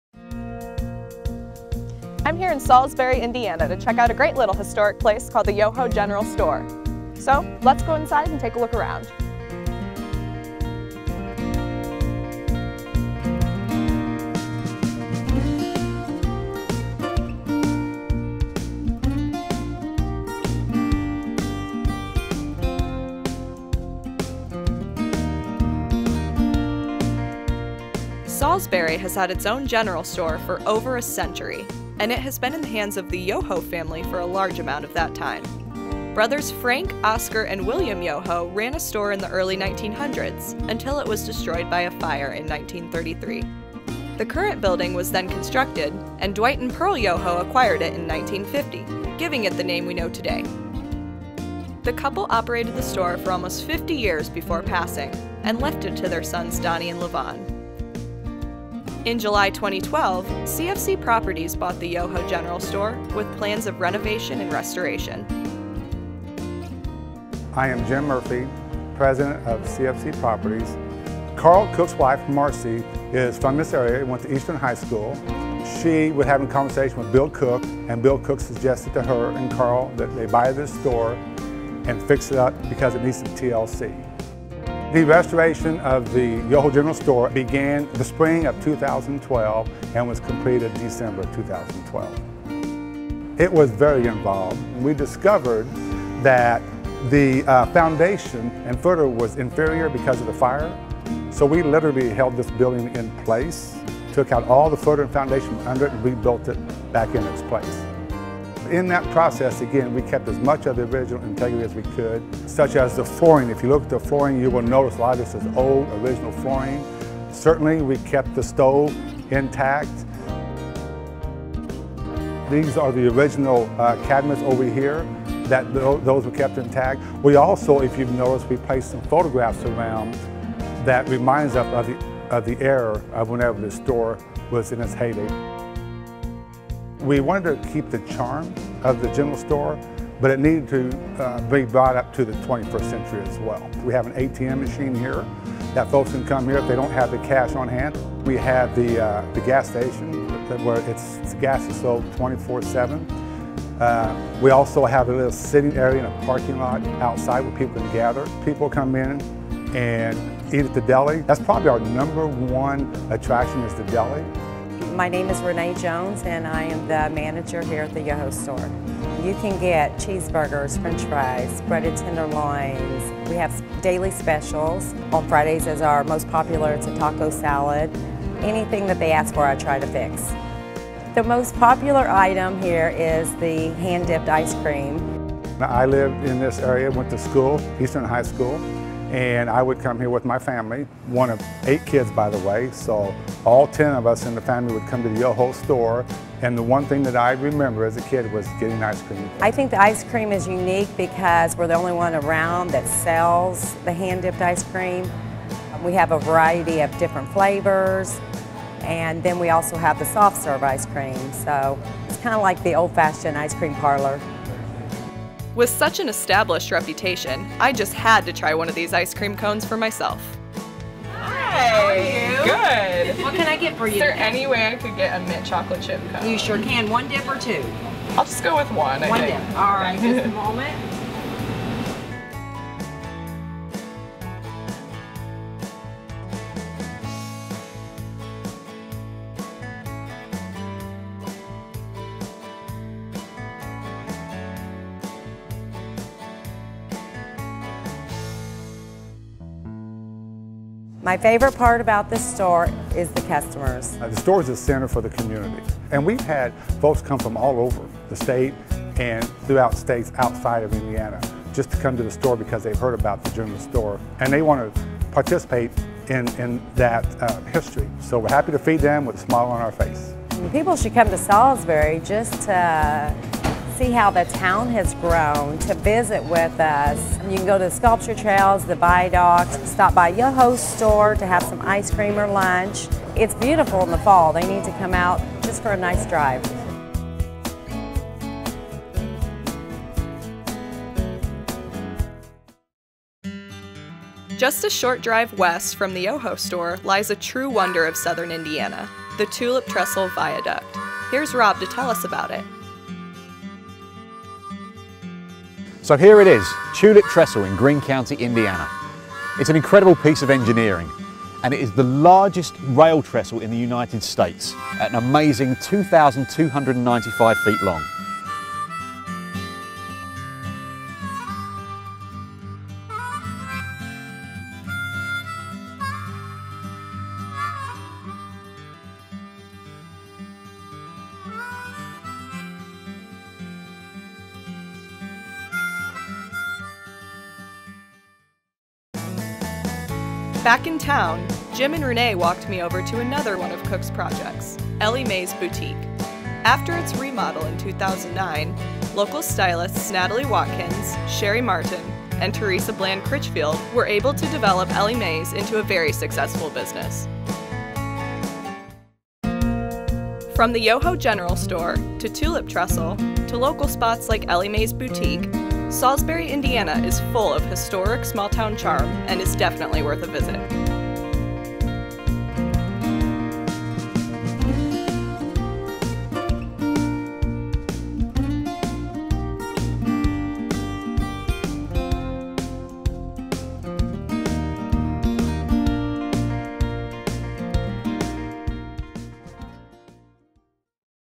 Smithville Feature - Yoho General Store, Solsberry Indiana
Lindsey visits the very unique Yoho General Store in Solsberry, Indiana.